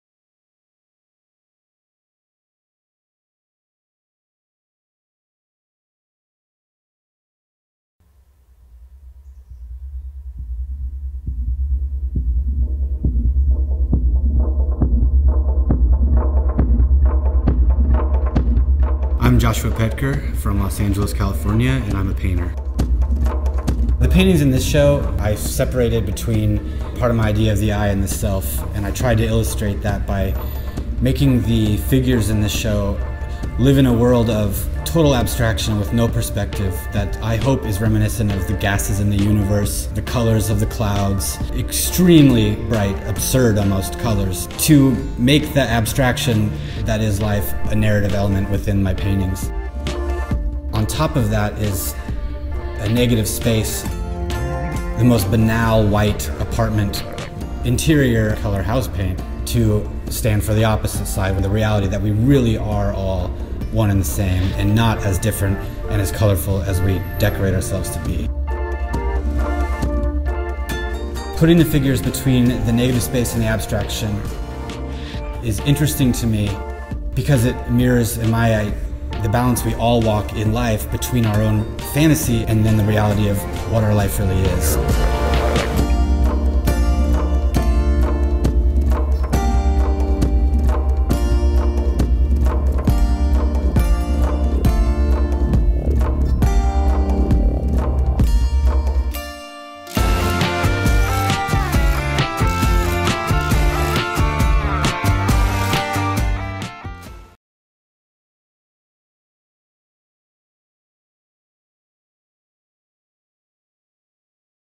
Artist Joshua Petker @ Shooting Gallery Colin Day Warholian
Joshua Petker
We're Not As Colorful As We Think We Are

We're Not As Colorful As We Think We Are, a solo exhibition by Los Angeles based artist, Joshua Petker. The new body of work continues the artist's ongoing exploration of color abstraction, negative space, and the human form. Petker explores the way humans use color to manipulate their physical appearance in order to transcend their reality. Through these new works, Petker's colors expose the desire for human metamorphosis all the while reminding us that underneath the layers of color, we are more alike than we may think.